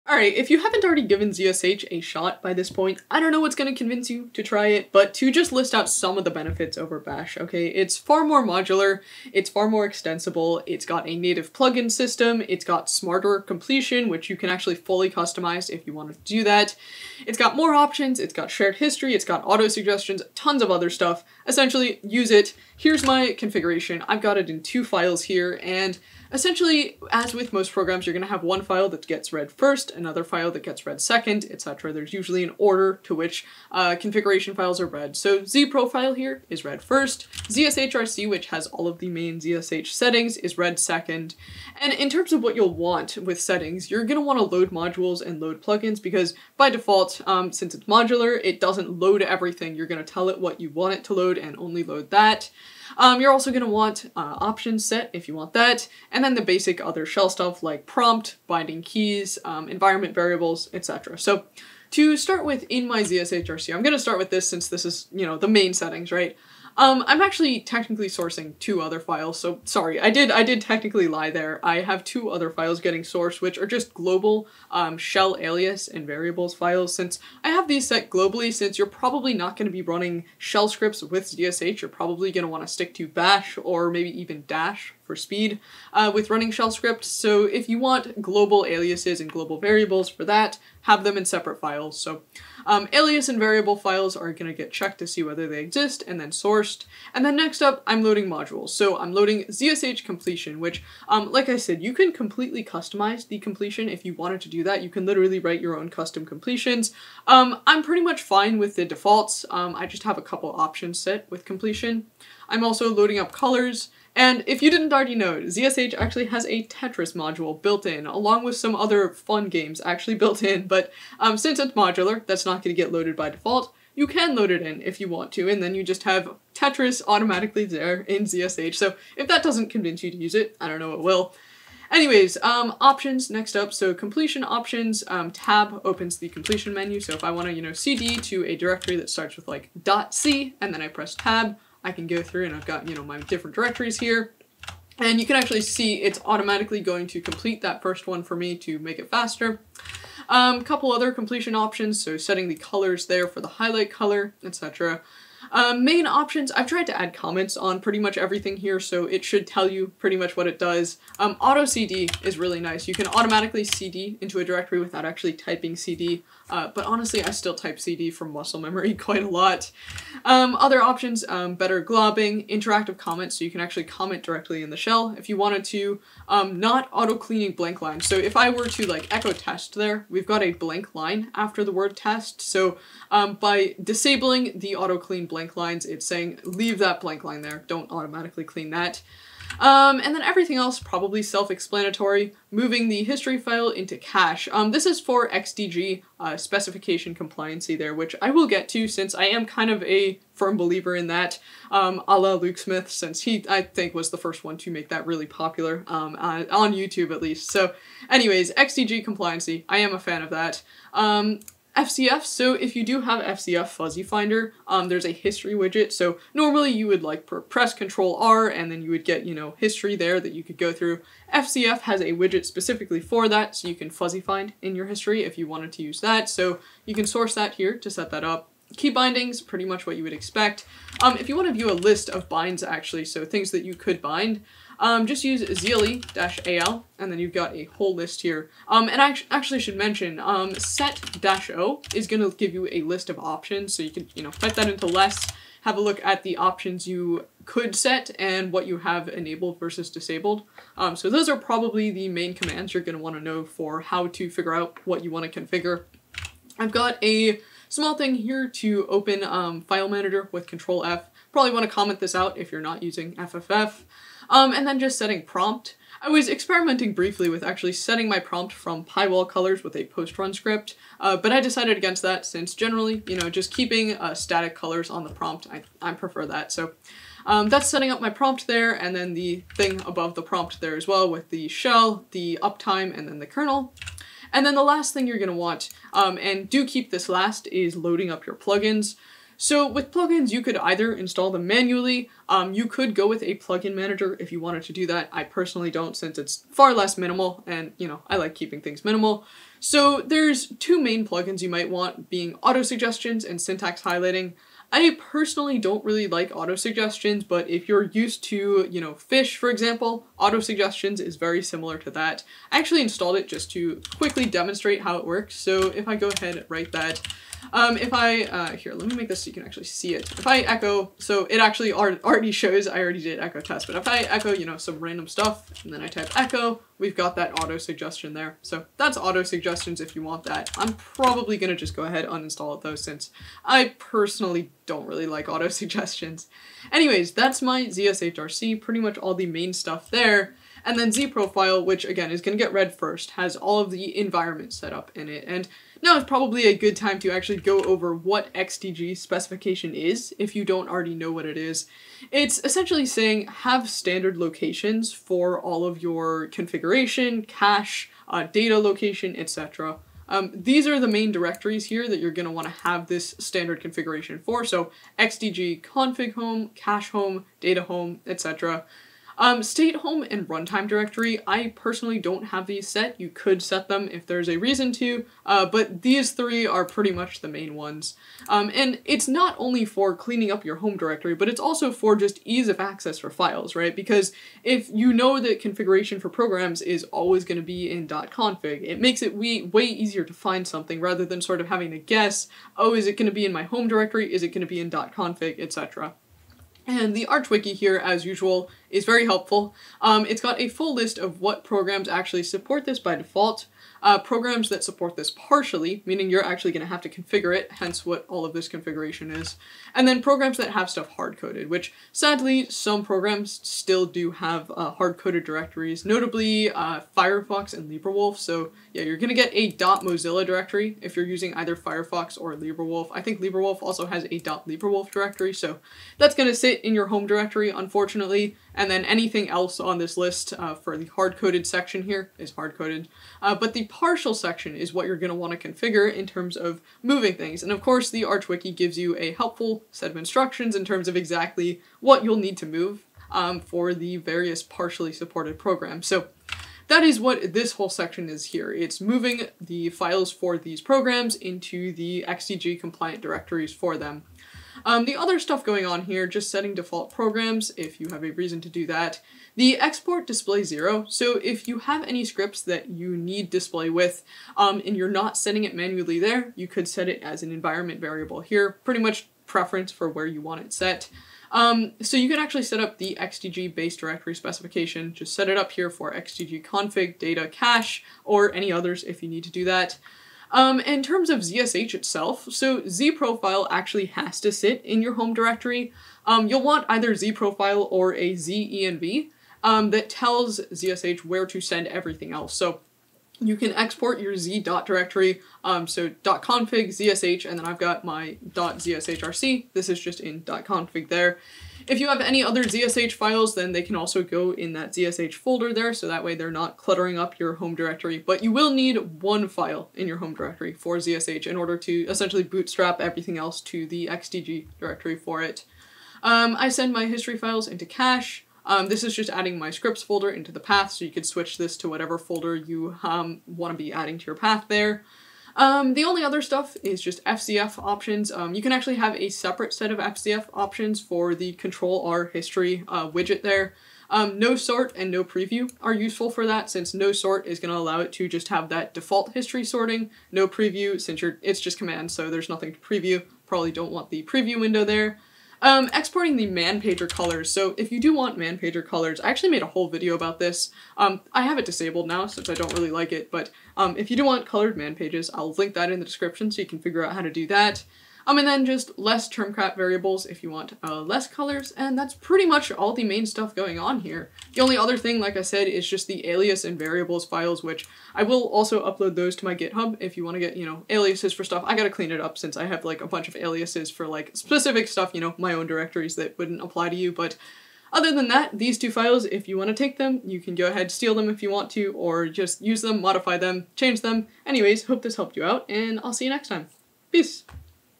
zsh vs bash: why i switched & how to set up the ultimate custom shell
The z shell (zsh) - a great alternative to bash for the modern *nix system :^)

Wallpaper photo by Ole Henrik Skjelstad, among the series of his photos of the cabin, titled "A Moment of Light".

0:00 .
0:23 files present, .zshrc
3:54 keybindings, options
4:25 setting prompt
4:46 zsh plugins (autocompletion, syntax highlighting, etc)
5:40 .zprofile, XDG spec